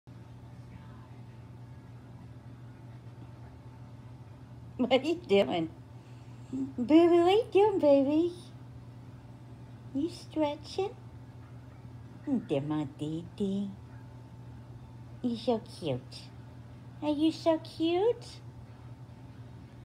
He loves showing off his belly 🤣🥰
He is so Silly but he was really just doing his morning stretches￼